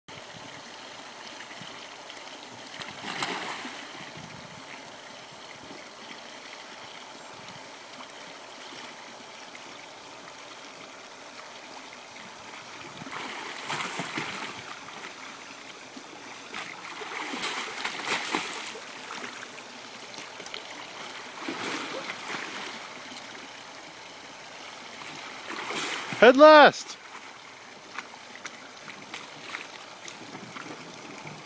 A Failed Kayak Roll
A friend trying to roll in my kayak.  He's attempting to bring his head up too soon and not quite sweeping far out enough.  Though the head is the larger problem